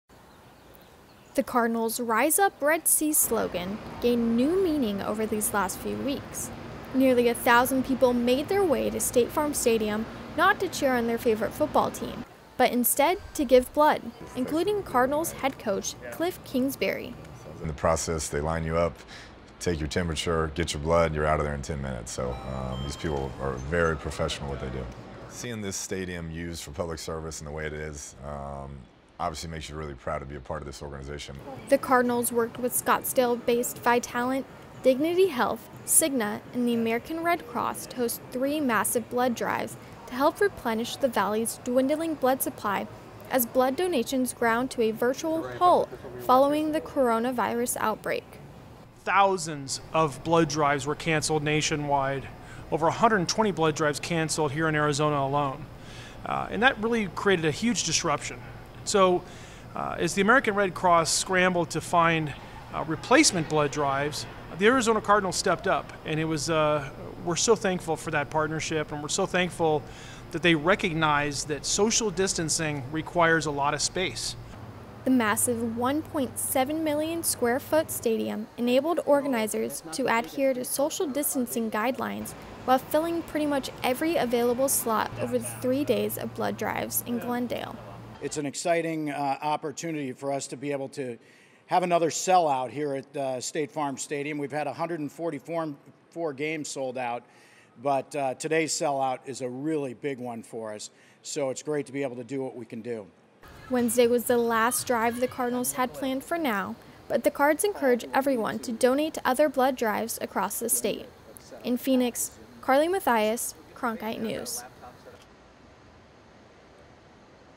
‘Red Zone’ at Cardinals blood drive | Cronkite News
The Arizona Cardinals have teamed up with Vitalant, Dignity Health, Cigna and the American Red Cross to alleviate blood shortages due to the COVID-19 pandemic.

Video by Karli Matthais/Cronkite News